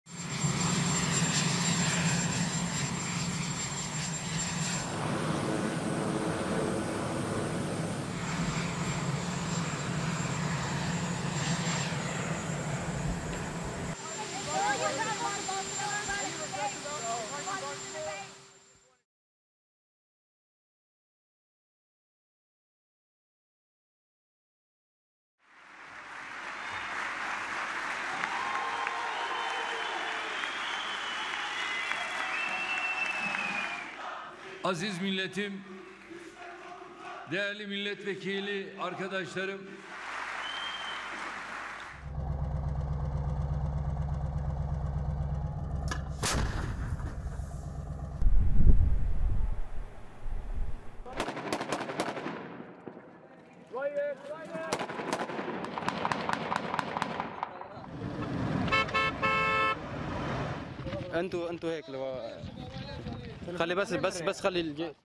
ترامب في رسالة لأردوغان: لا تكن متصلباً ولا أحمقاً.. وإلا فاقتصادك إلى دمار
وبُعث خطاب ترامب يوم 9 أكتوبر – تشرين الأول وهو نفس اليوم الذي بدأت فيه عملية "نبع السلام" التركية في سوريا وطالب فيها ترامب إردوغان بالتوصل لاتفاق يجنب تركيا "ذبح الآلاف من الناس".…